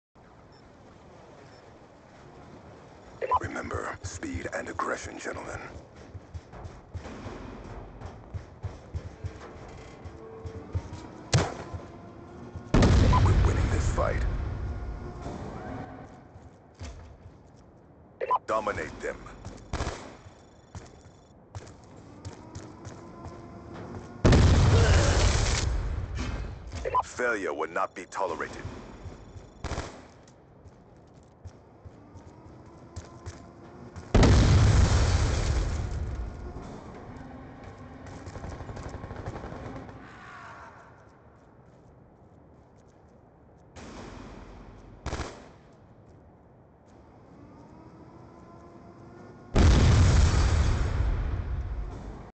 First blood grenade launcher
at the binning of each game, if i have a grenade launcher, i fire it across the map in hopes to get a kill. this time i got lucky and got one whilst also confusing another